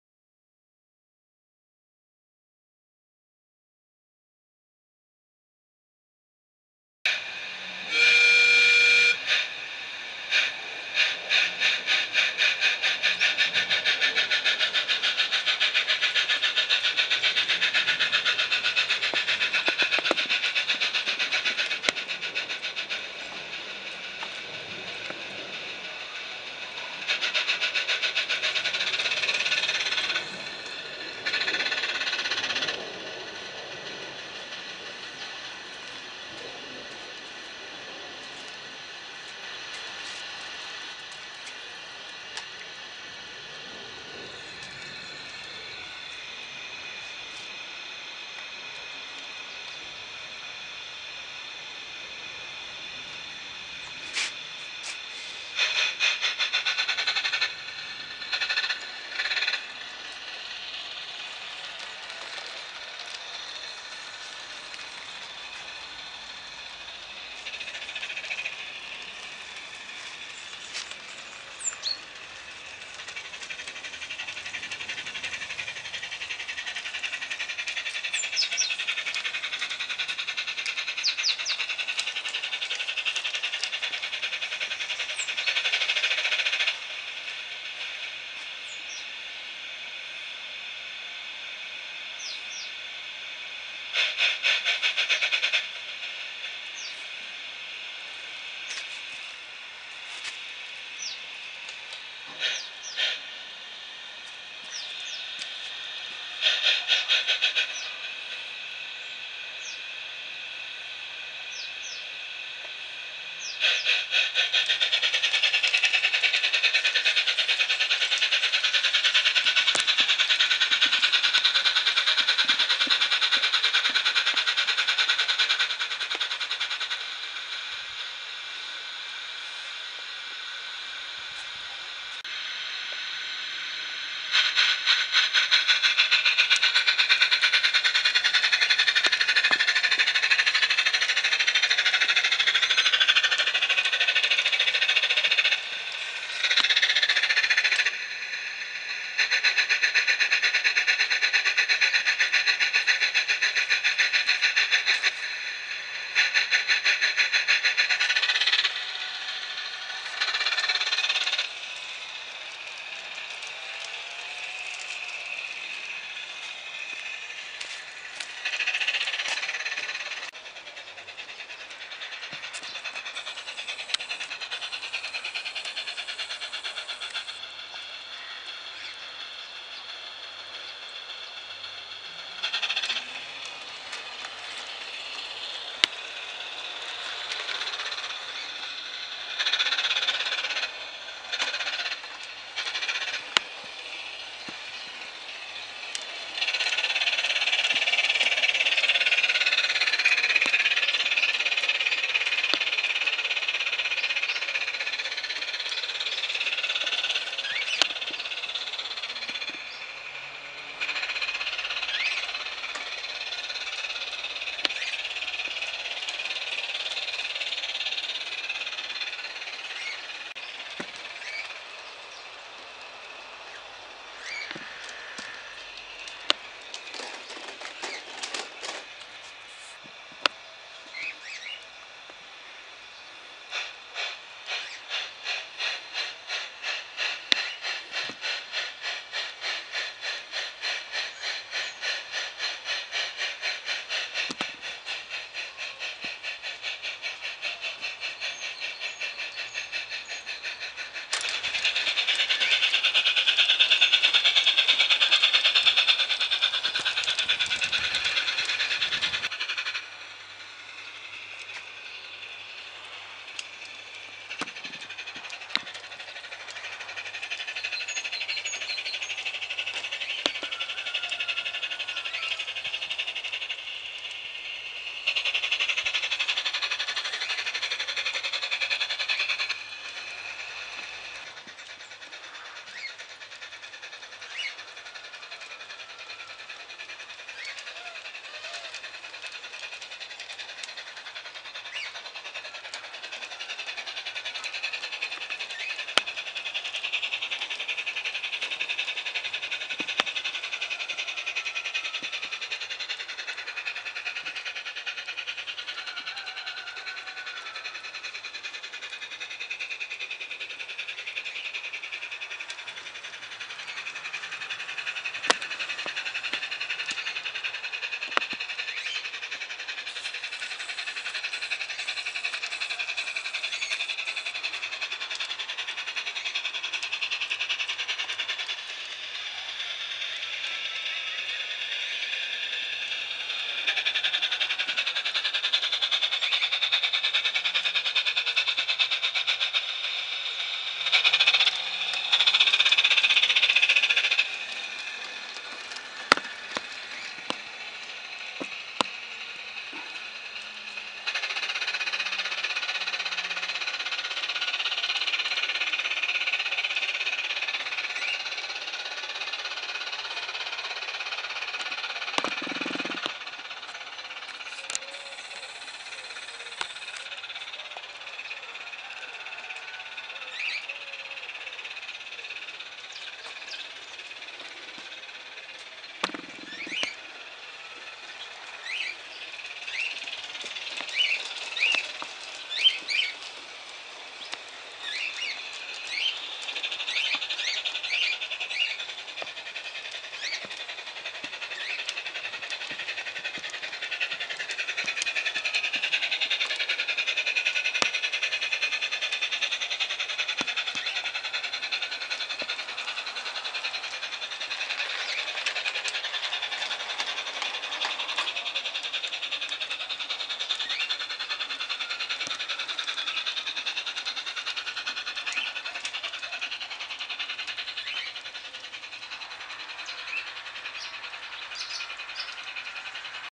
R.K.V.t.
Az alpszitu : 4 helyen cserélve az R1 ív R2re. Ami mindenféle képen jót tesz a mozdonyoknak. A bejárás egy olyan mozdonnyal , ami még soha nem ment körbe a pályán. Nos a Harzernak is vannak , voltak gondjai. Futókerék tengelynyomás, vonó szerkezet , stb. De két órás szarakodás és közbenső pálya finomítás után , csak körbe lehetett vonatozni gond nélkül. de hát ez van,. ez egy ilyen sport.